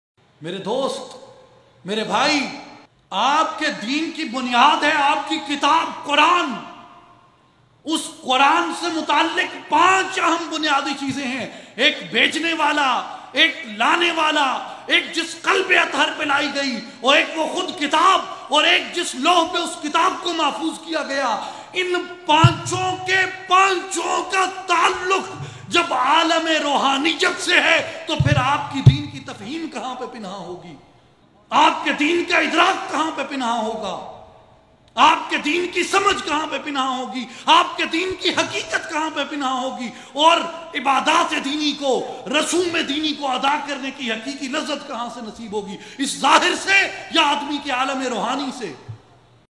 Five important things about Quran.
[ Explained By: His Excellency Sahibzada Sultan Ahmad Ali Sb ]
In annual Melad e Mustafa SAWW & Haq Bahoo Conference, organized by Islahee Jamaat & Aalmi Tanzeem ul Arifeen, at Gujranwala.